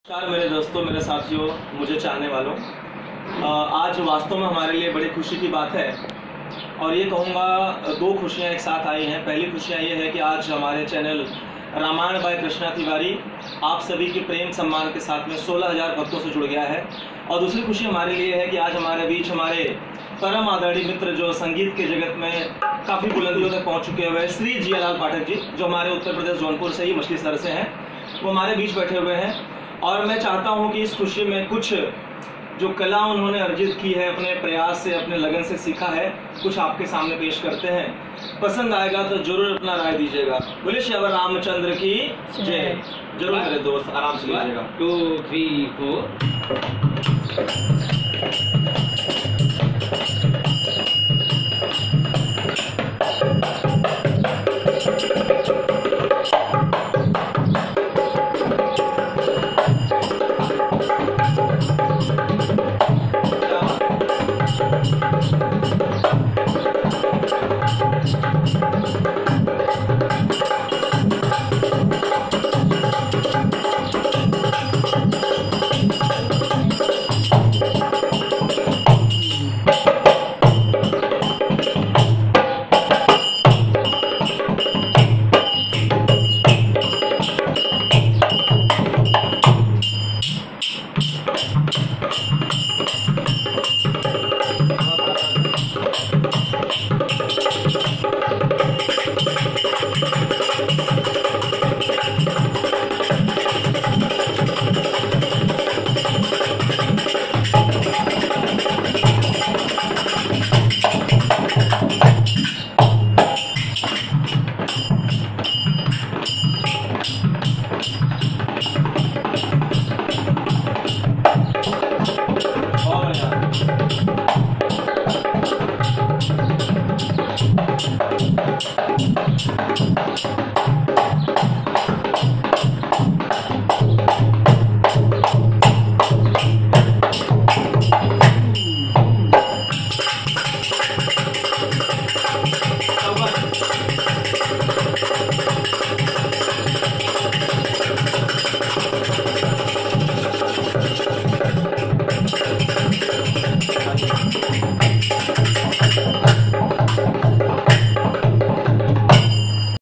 🙏धन्यवाद🙏आप के अपार स्नेह के लिए:- बेतरीन ढोलक, ताल और लय का अनोखा संगम Ramayan By Krishna Tiwari
आप के खुशी के लिए अग्रसर आप का यह यूट्यूब चैनल 16 हजार भक्तो से जुड़ गया है उसी की खुशी और आप का आभार प्रकट करने हेतु यह वीडियो आप सभी को धन्यवाद रूप में सप्रेम भेंट।

ढोलक, तबला उस्ताद :- श्री जियालाल पाठक मोबाइल नंबर - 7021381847, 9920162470